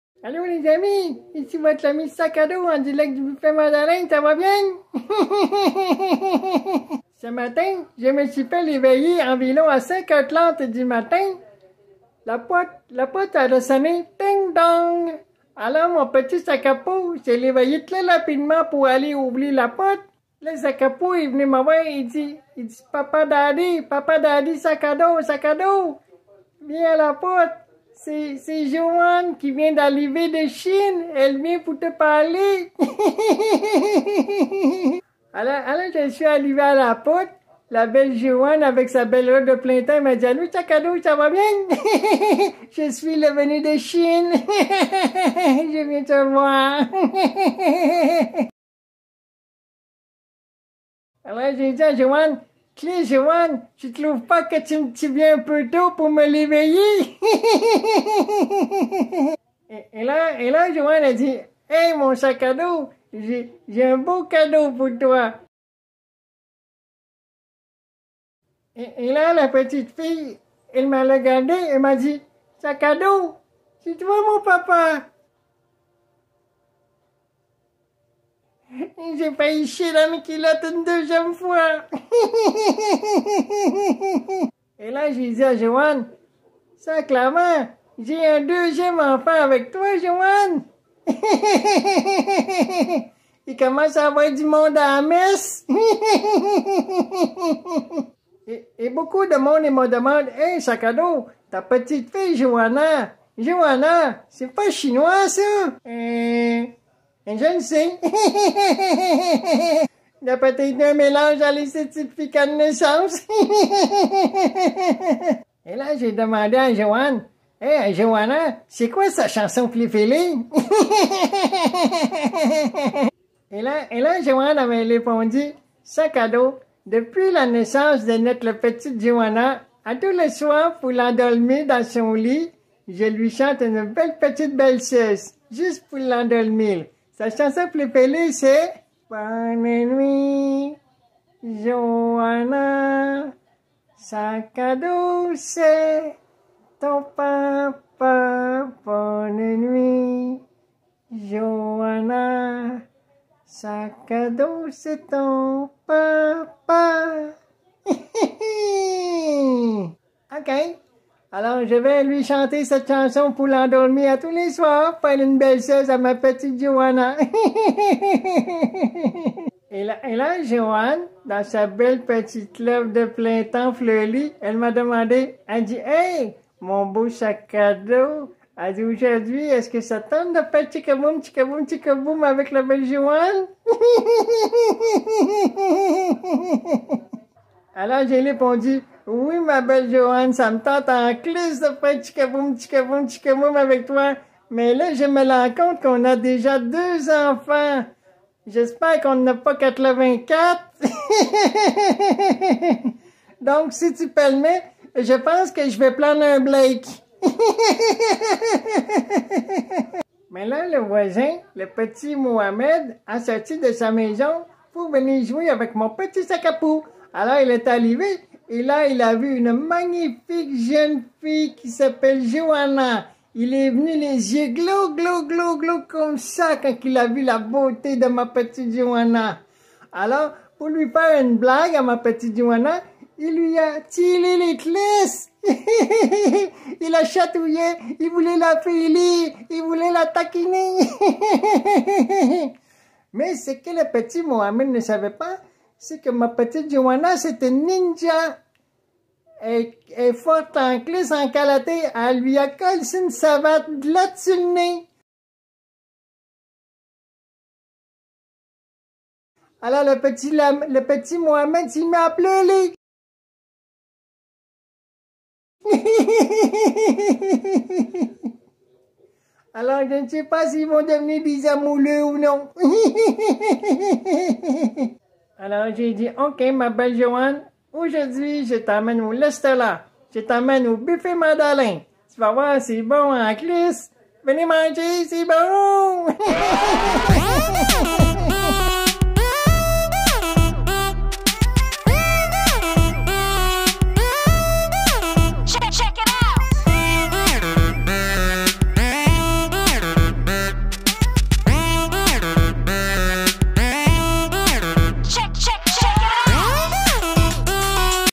Sakado (17)
Sakado se fait réveiller par Jowanne à 5h30am...

La première apparition de Sakapou dans: Sakado (14)...

L'arrivée de Sakapou dans:
Sakado (15)...

Première apparition du frère à Sakado dans:
Paul L. (18)...